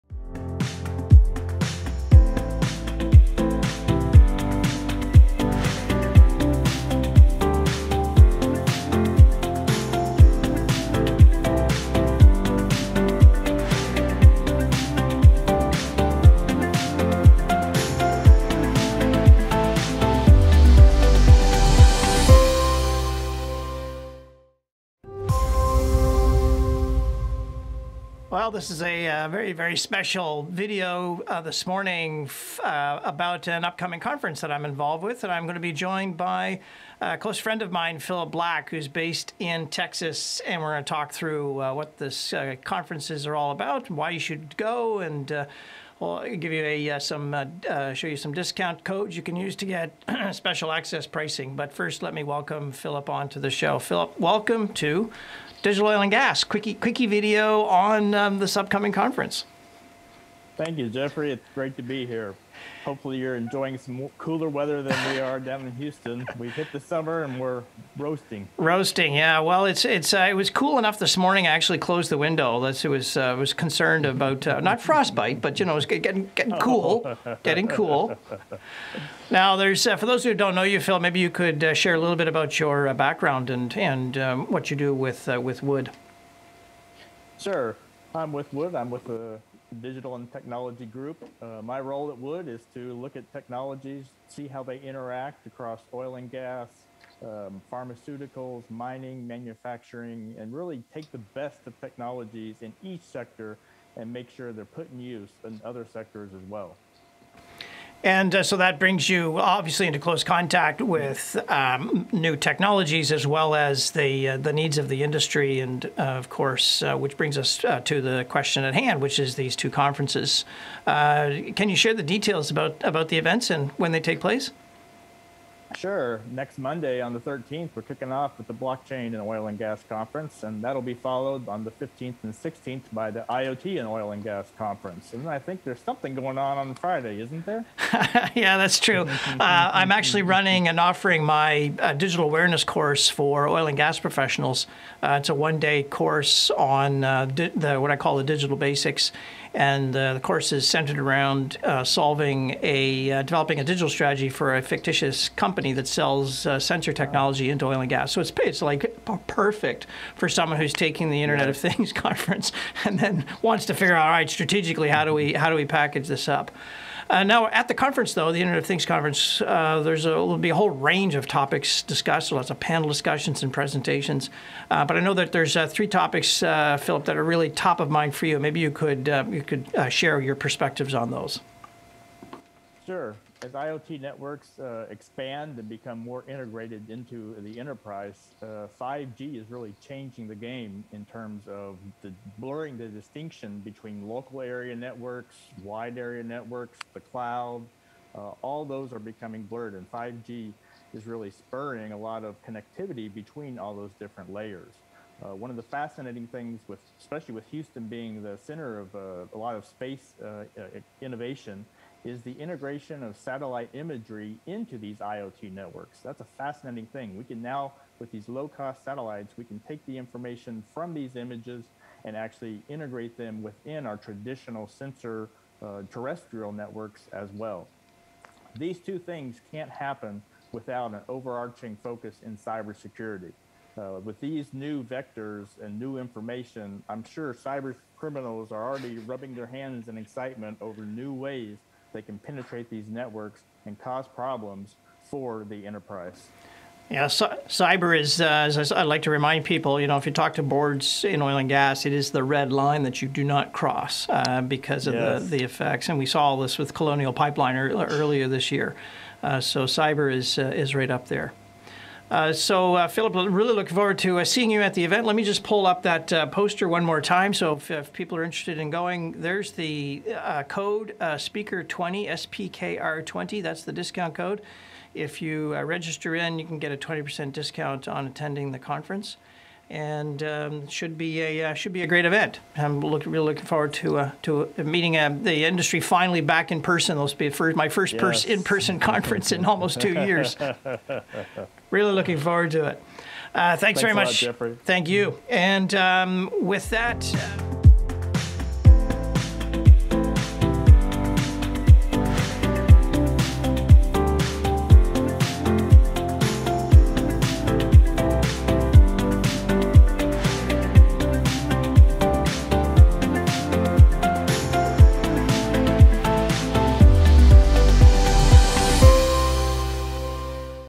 IoT and Blockchain Conference Teaser with Philip Black
Philip Black of Wood, and I are both attending the IoT in Oil & Gas Conference, the world’s largest focused IoT in Oil & Gas event, in-person this year, September 15 and 16, 2021 in Houston.

Register here ==

This conference brings together the community of stakeholders, including operators, service companies, EPCs, drilling companies and technology solution providers, to debate the crucial challenges, issues and emerging trends shaping the digital transformation of oil & gas.

The imperative of decarbonization as well as the dual-shocks of the global pandemic and oil price collapse has increased the urgency for operators to fast-track their  digital transformation programs.

Automation, sensors, digital twins, AI, drones - all reduce operating costs and aide in reducing carbon footprints, crucial to surviving the present and thriving in the future.